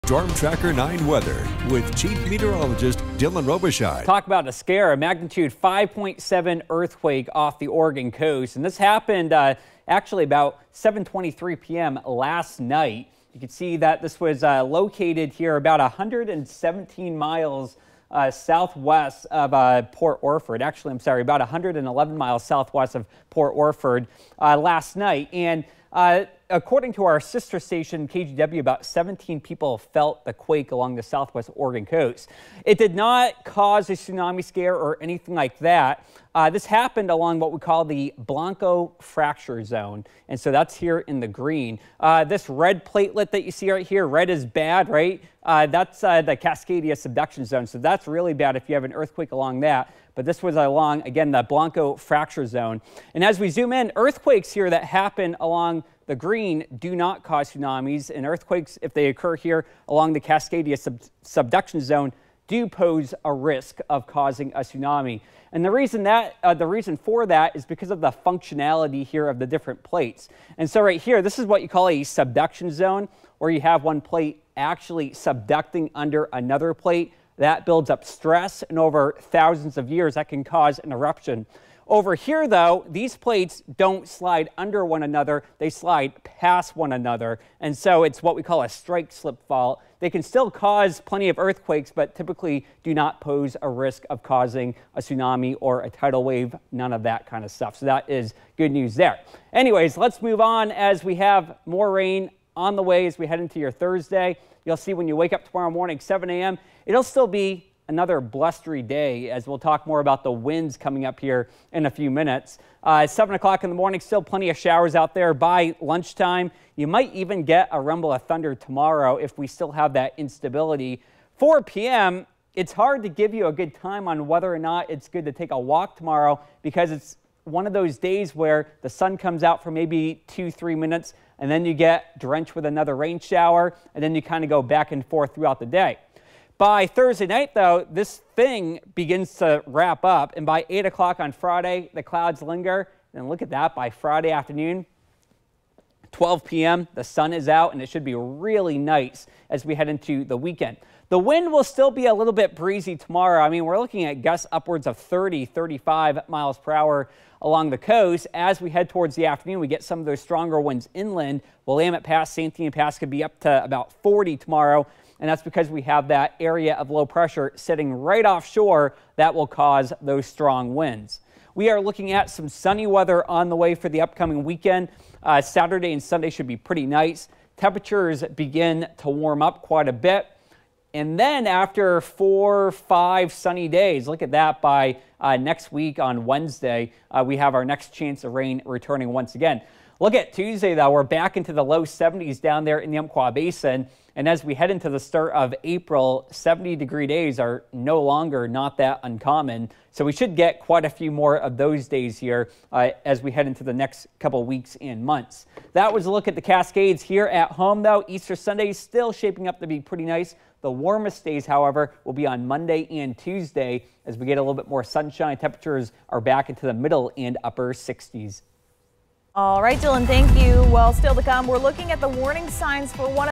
Magnitude 5.7 earthquake reported off Oregon coast
Here's why these types of earthquakes don't typically cause a tsunami.